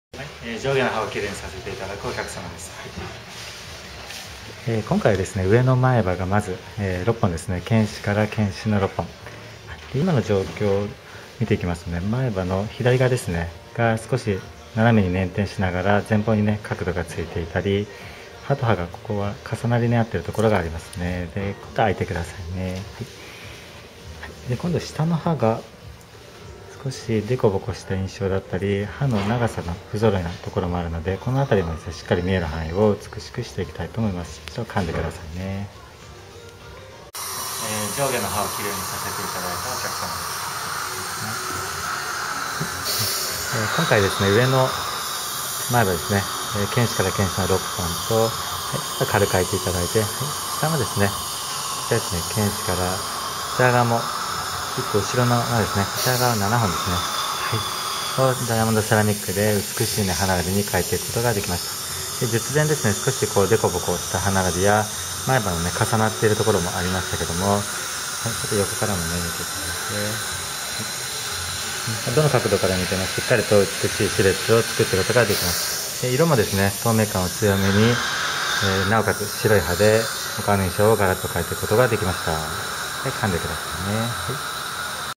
☆湘南美容歯科・名古屋院☆ 前歯の歯並びを上下で美しく♪
30代男性

上の前歯の重なって生えているところや下の歯のガタガタ歯並びを、短期間のセラミック治療で大きく改善していきました！

歯並びや、金属入りのさし歯のやり変え、虫歯などでお悩みの方は湘南美容歯科・名古屋院まで是非ご相談下さい♪

無料カウンセリングでお待ちしております。